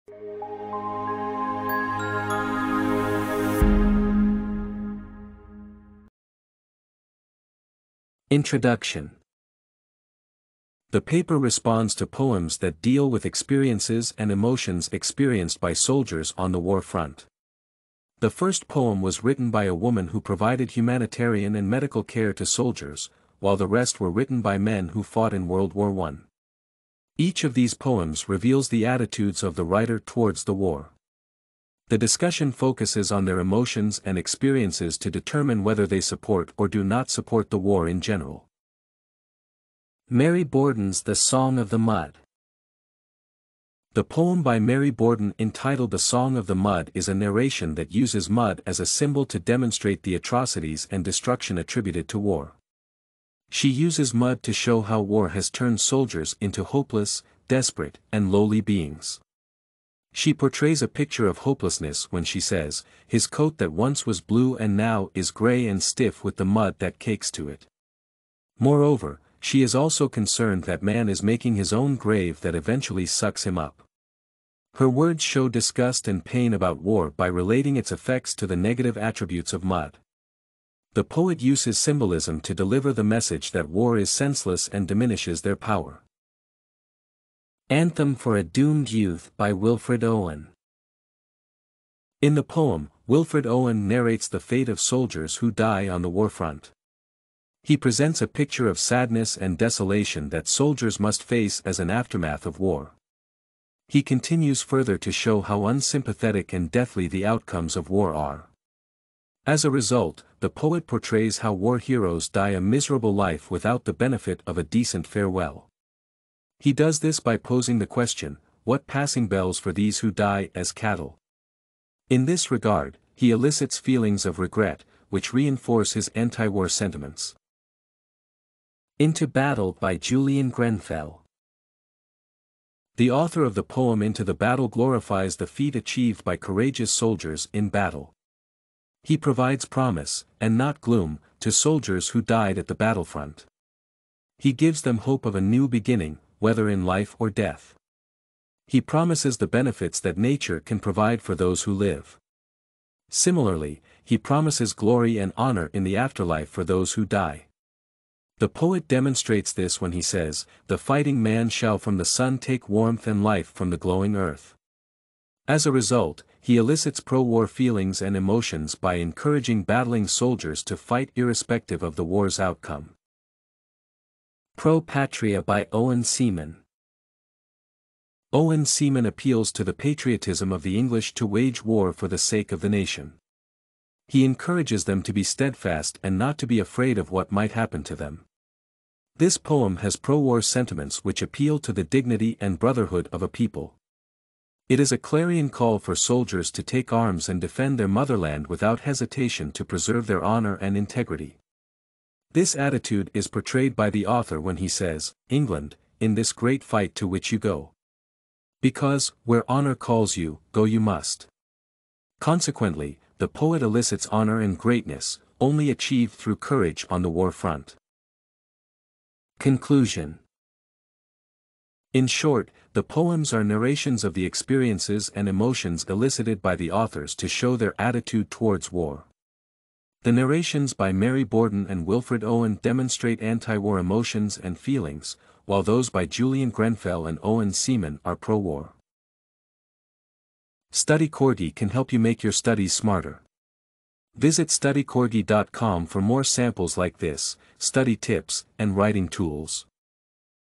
Poems About War by Borden, Owen, and Others - Research Paper Example
📄 Research Paper Description: This research paper responds to poems that deal with feelings and emotions experienced by soldiers on the war front.

✔️ Topics: Literature, War
✔️ Language: 🇺🇸 English
✔️ Words: 602

🔍 References:
Borden, Mary. The Song of the Mud. All Poetry.
Grenfell, Julian. Into Battle. All Poetry.
Owen, Wilfred. Anthem for a Doomed Youth. All Poetry.
Seaman, Owen. Pro Patria. All poetry.